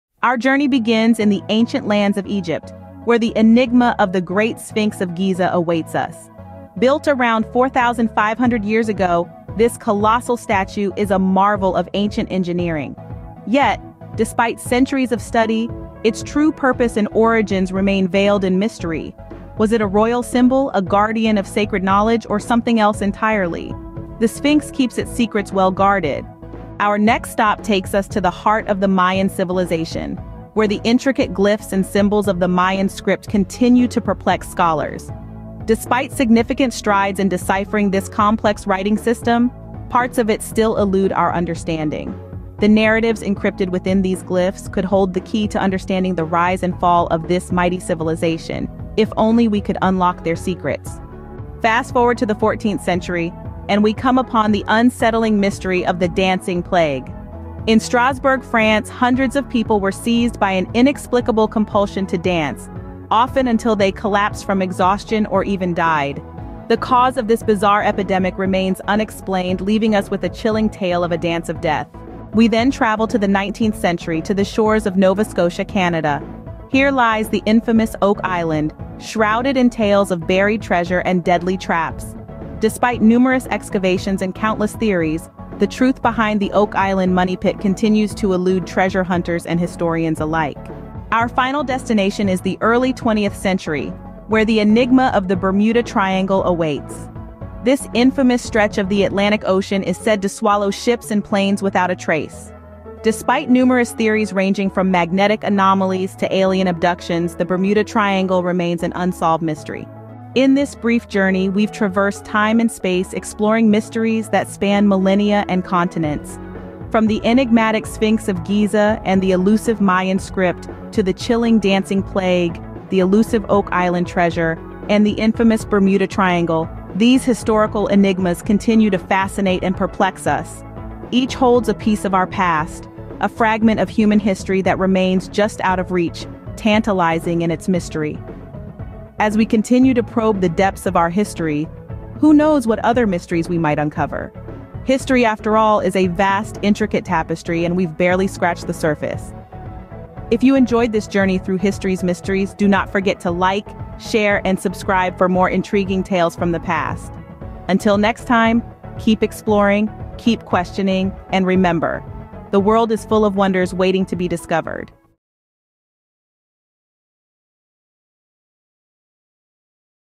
Decoding History's Enigmas: Unraveling the Mysteries of the Past
captivating journey as we delve into the unsolved mysteries and intriguing puzzles that have perplexed historians and archaeologists for centuries, seeking answers to age-old questions.